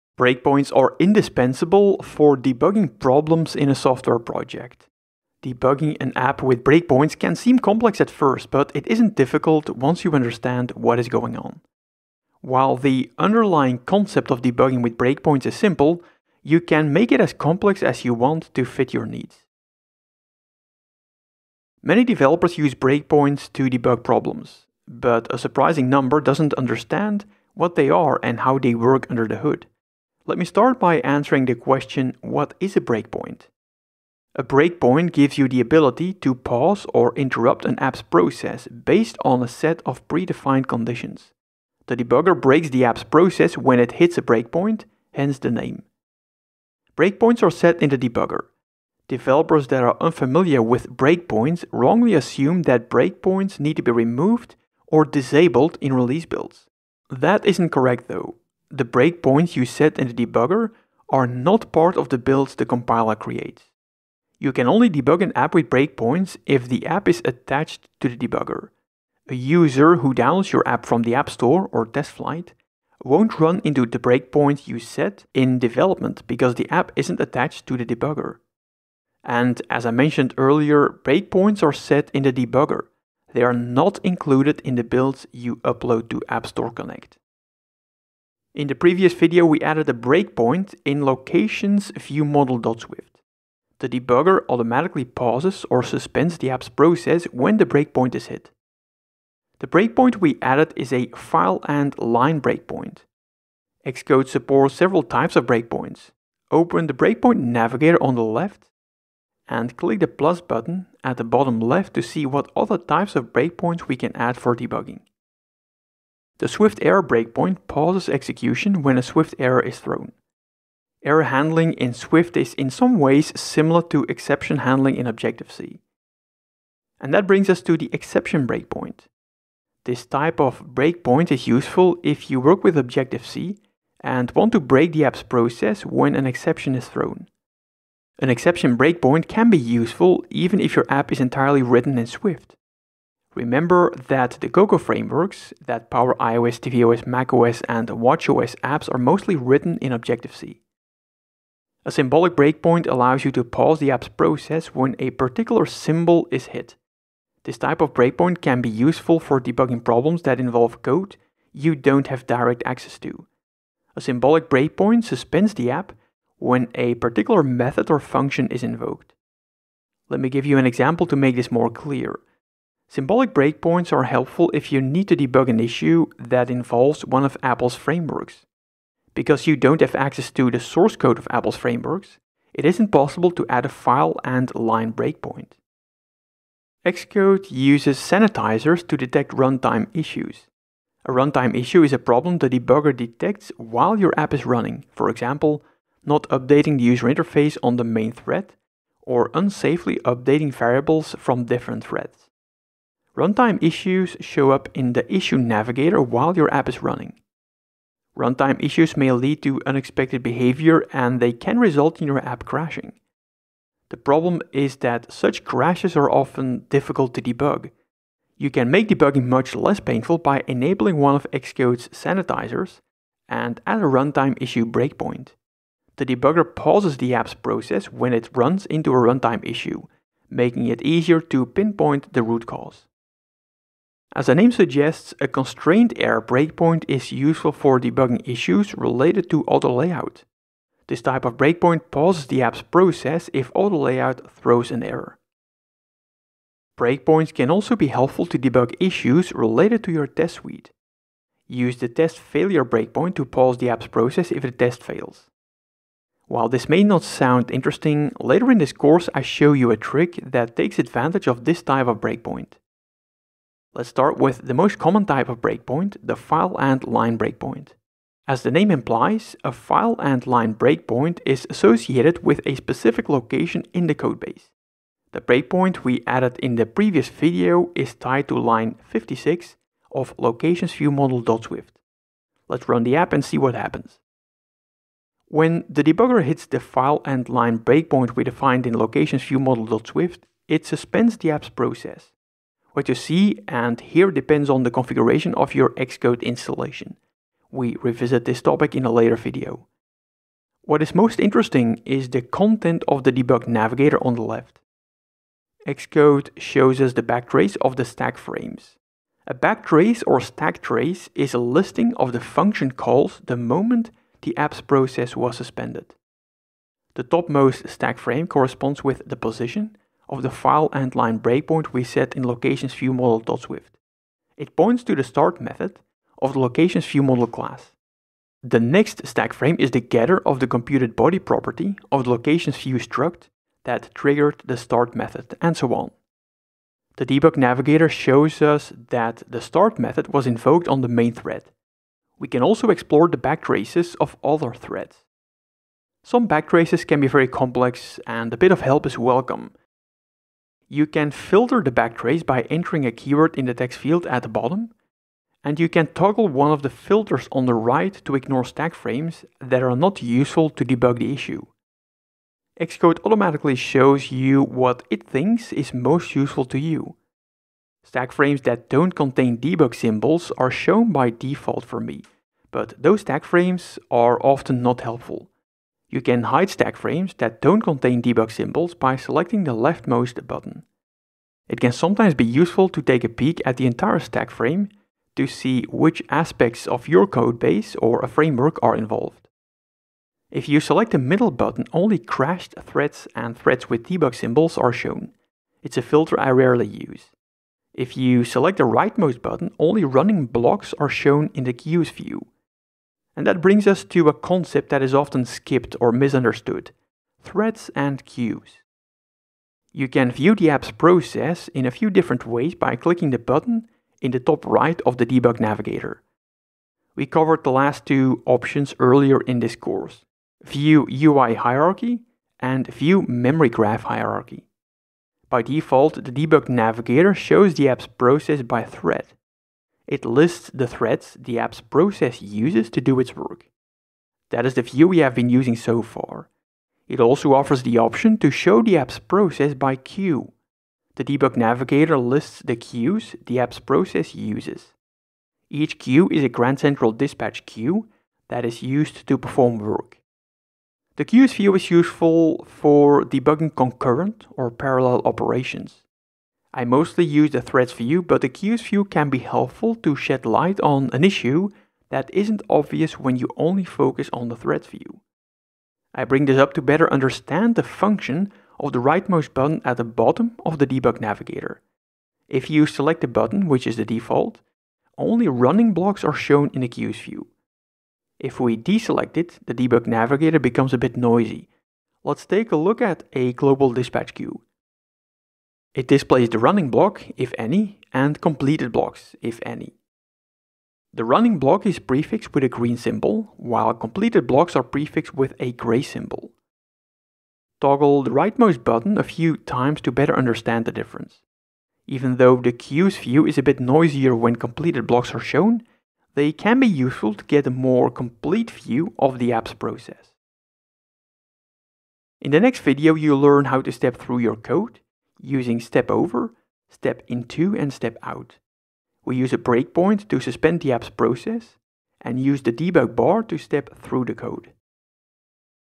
Debugging Apps with Xcode 15: Debugging with Breakpoints
Breakpoints are indispensable for debugging problems in a software project. Debugging an app with breakpoints can seem complex at first, but it isn't difficult once you understand what is going on. While the underlying concept of debugging with breakpoints is simple, you can make it as complex as you want to fit your needs.

Many developers use breakpoints to debug problems, but a surprising number doesn't understand what they are and how they work under the hood. Let me start by answering the question what is a breakpoint?

A breakpoint gives you the ability to pause or interrupt an app's process based on a set of predefined conditions. The debugger breaks the app's process when it hits a breakpoint hence the name.

Breakpoints are set in the debugger. Developers that are unfamiliar with breakpoints wrongly assume that breakpoints need to be removed or disabled in release builds. That isn't correct, though. The breakpoints you set in the debugger are not part of the builds the compiler creates.

You can only debug an app with breakpoints if the app is attached to the debugger. A user who downloads your app from the App Store or TestFlight won't run into the breakpoints you set in development because the app isn't attached to the debugger. And, as I mentioned earlier, breakpoints are set in the debugger, they are not included in the builds you upload to App Store Connect.